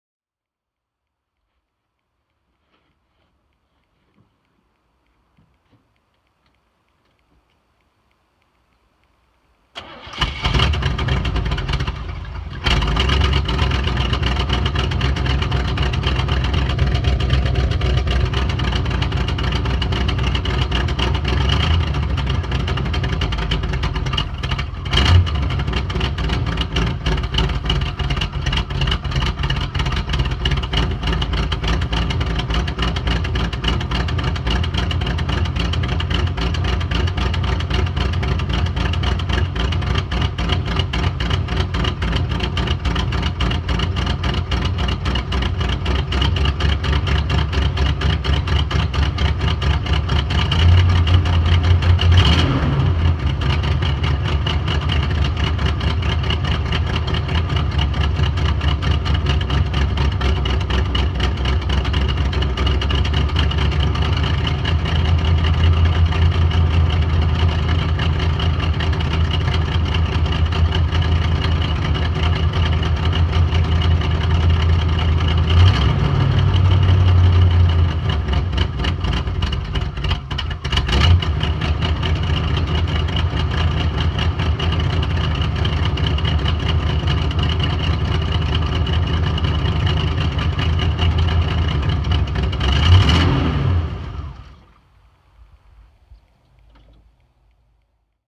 1953 Studebaker Start
July 30, 2016:
Only yesterday received my Dad's cars after a year long struggle with a fraudulent shipping company.  It's worse for wear with all the sitting around and neglect. The Idle is bad and needs good tuning. (There is a bumpy cam in here, but still not happy)
Will be making other videos of the results as time goes.